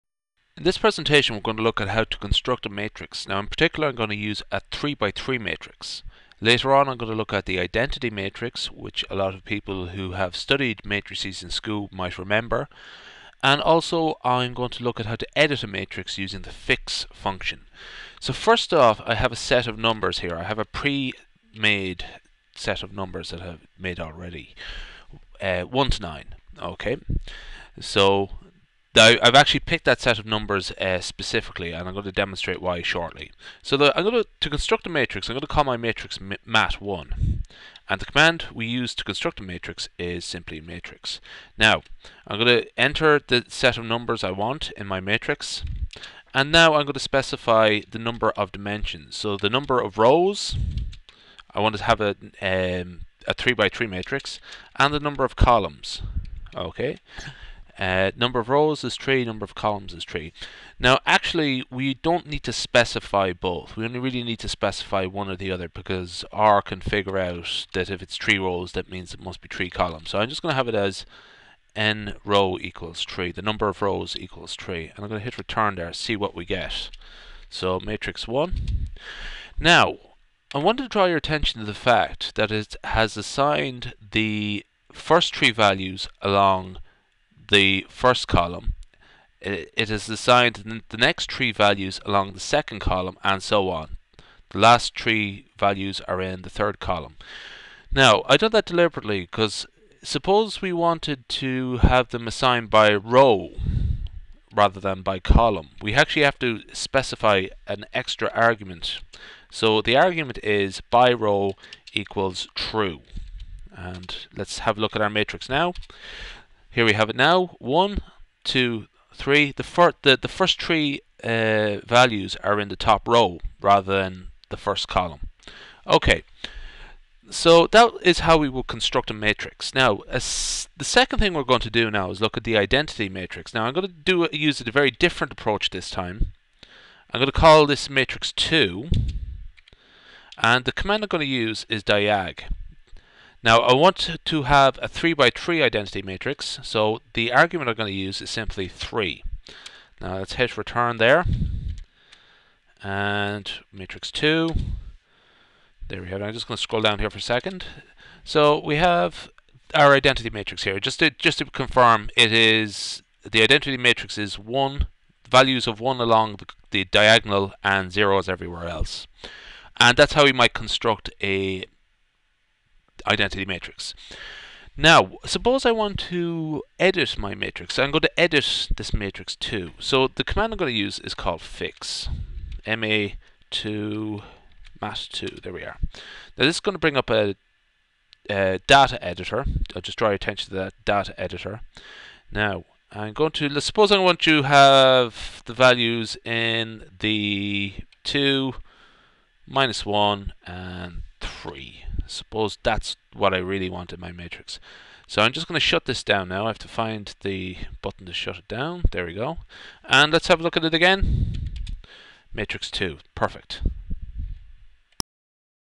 R Tutorial : Creating a Matrix
Creating a matrix using the "matrix" function. The "diag" and "fix" functions are also briefly discussed.